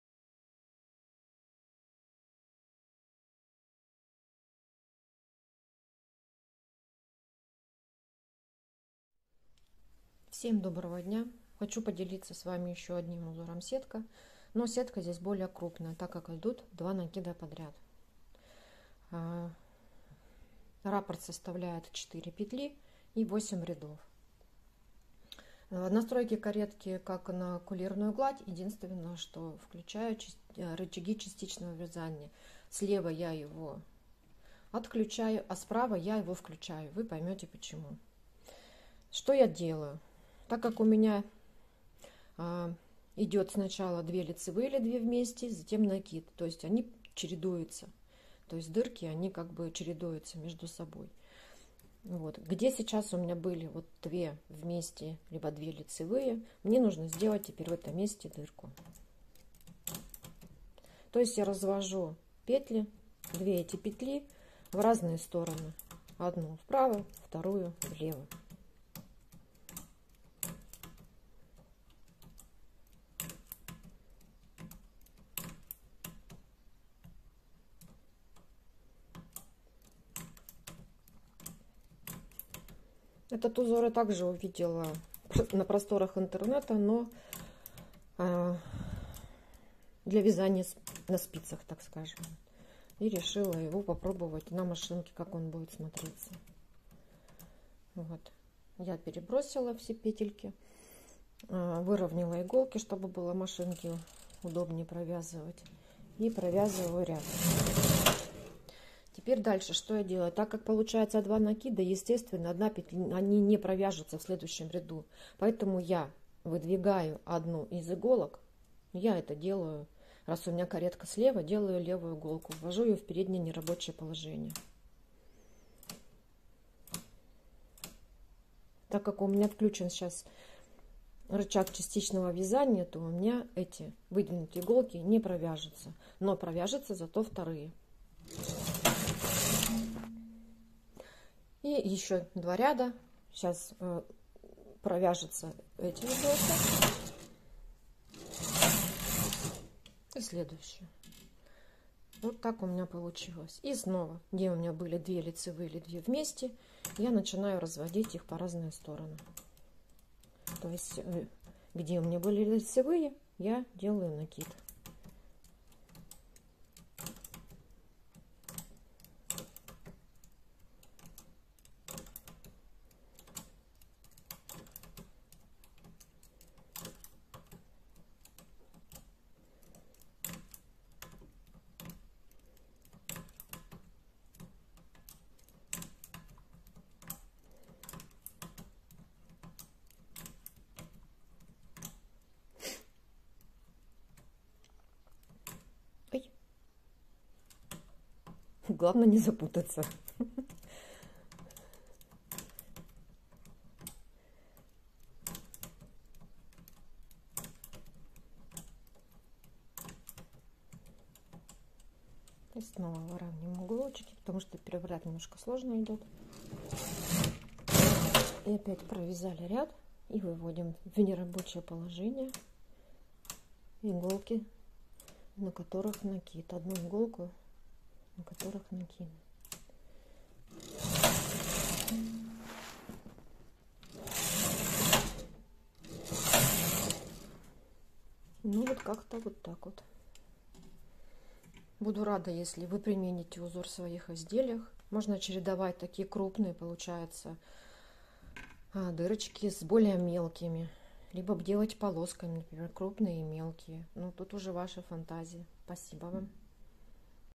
Узор 3 - сетка с двойными накидами. Машинное вязание.
Ещё один вариант сетки. Вяжется она с двойными накидами.
         В видео я показываю, как провязать на машине два накида подряд.
         Узор взят из узоров для вязания на спицах и переложен на машину.
          Его можно использовать при вязании любых изделий летних, демисезонных или зимних.
         Можно дырочки расположить по вашему дизайну, увеличить петель между дырочками, увеличить ряды, сделать их не в шахматном порядке, а друг над другом, или совсем редко расположить по изделию или расположить вертикальными и горизонтальными. А можно скомбинировать их с вариантами другой сетки. У меня  на канале можно посмотреть другие варианты сеток.

В общем большой полёт для творчества.  И я хочу пожелать вам творческих успехов.